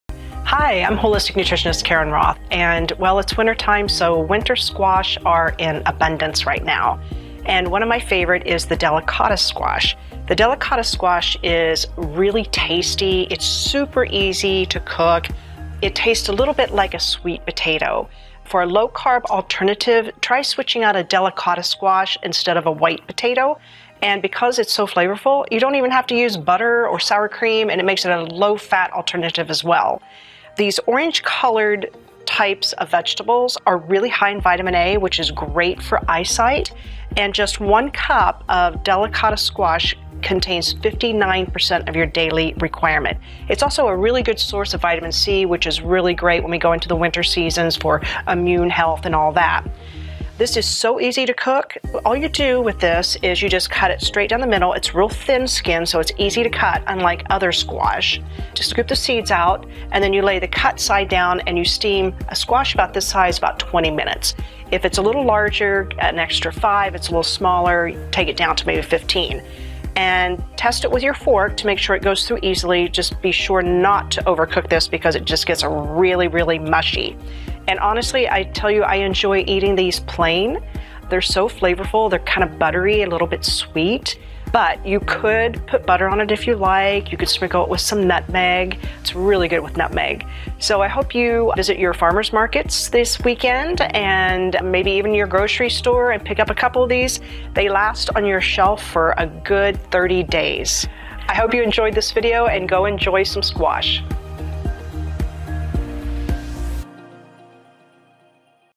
The Health Benefits of Delicata Squash
Delicata squash is an easy-to-prepare dish packed with powerful nutrients. Holistic Nutritionist Karen Roth, MSNC breaks down the health benefits.